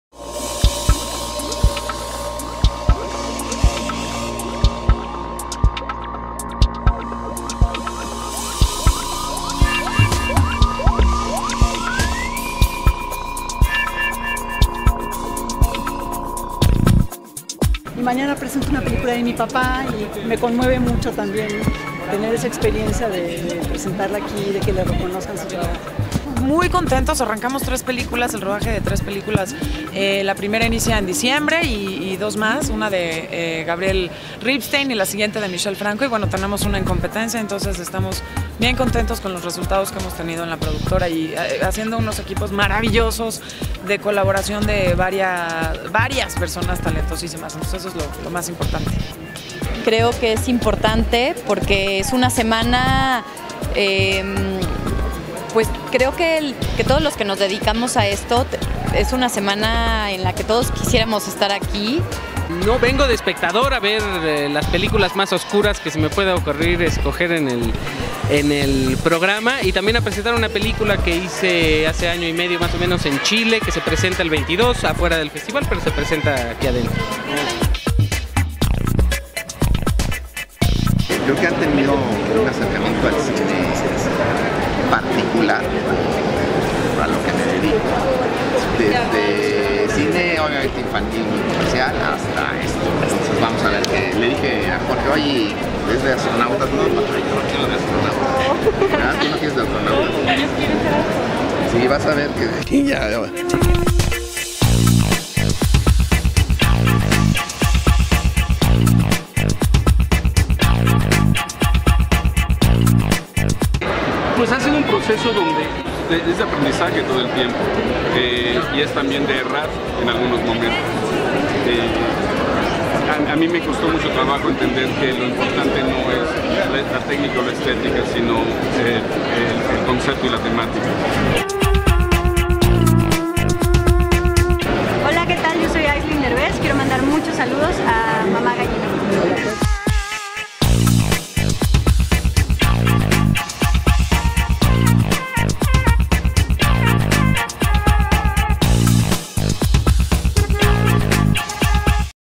Alfombra roja FICM #11
Morelia se viste de gala para la inauguración de la 11a edición del Festival Internacional de Cine de Morelia, contando con la presencia de Alfonso y Carlos Cuarón, Eugenio Derbez, Arcelia Ramirez, Tessa Ia, Martín Altomaro, entre otros.